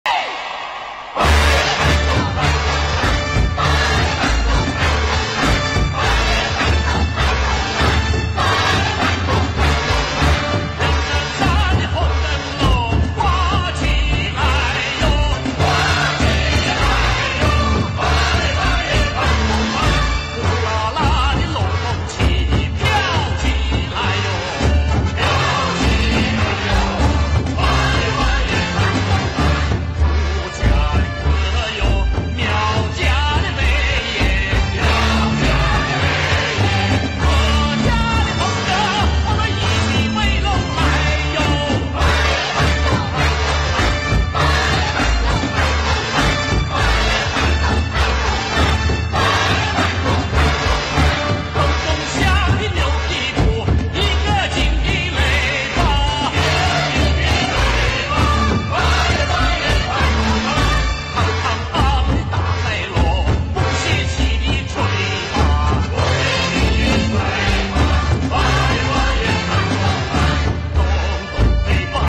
伝統の手振り踊りで「国際子どもの日」を祝う 湖北省
中国湖北省恩施トゥチャ族ミャオ族自治州来鳳県はトゥチャ族に伝わる祭祀舞踊、手振り踊りの発祥地の一つ。同県では、手振り踊りが毎年の「国際子どもの日」（６月１日）関連のイベントで欠かせない演目となっている。